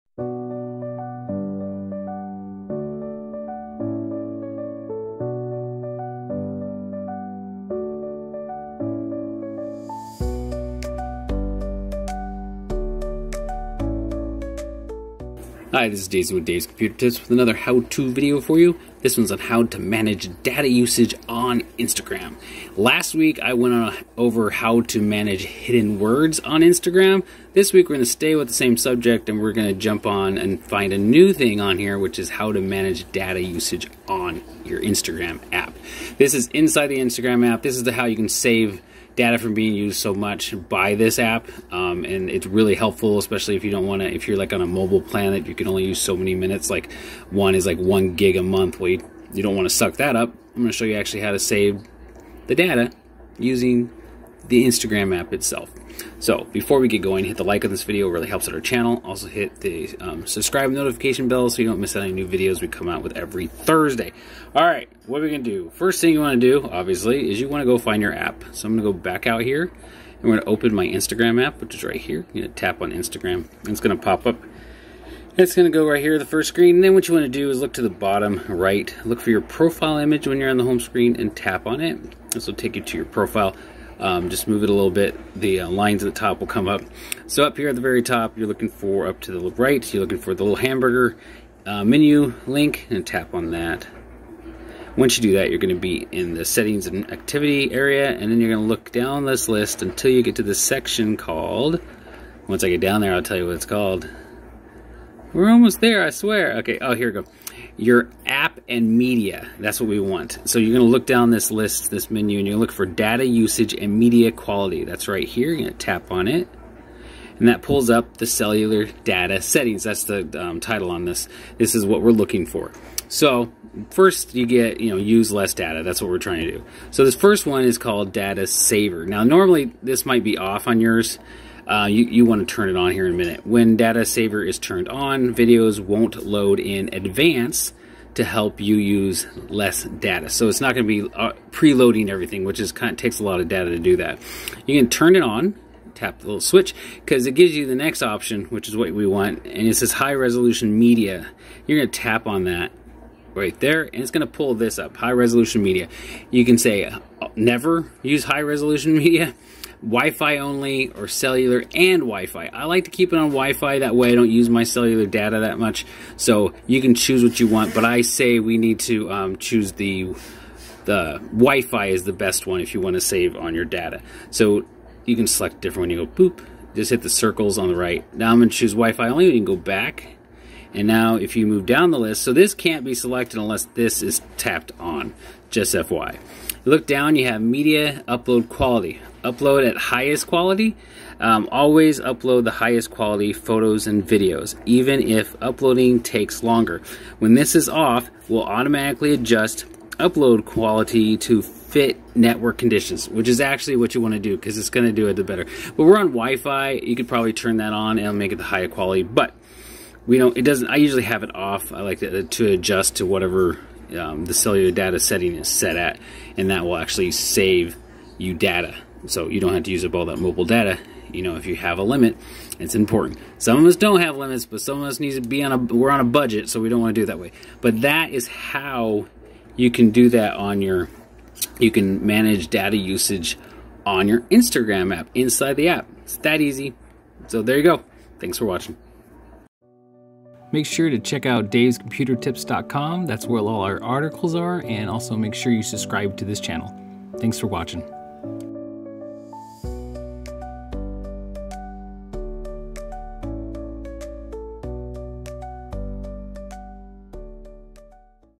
How To Manage Data Usage On Instagram
In this video I will show you how to manage your data usage on Instagram.

I’m back with another How-To on the social media platform, Instagram. Last week, I covered How To Manage Hidden Words. If you missed that, you can see it here:

Now that you have that knowledge, let me show you something new! Some of us need to watch our data usage to keep the bill lower. In this article, I want to show you how you can save that data from being used so much by this app. All apps like to use as much data as they can. Here I will show you how to limit it inside the app itself.

It’s time to start our class, so grab your devices, pencils, or whatever you need to learn.